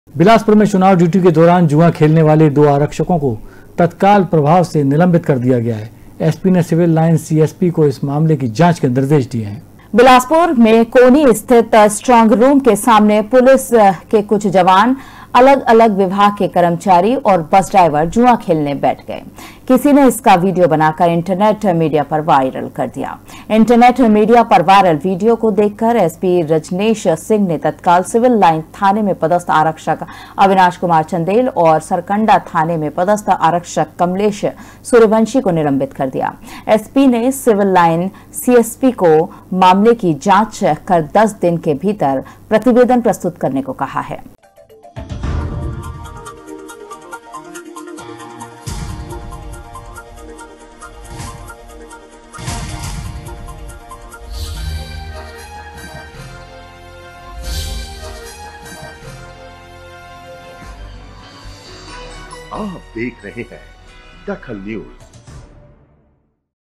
DAKHAL NEWS एसपी ने दो आरक्षकों को किया निलंबित
एसपी ने दो आरक्षकों को किया निलंबित
सोशल मीडिया पर वायरल हुआ  जुआ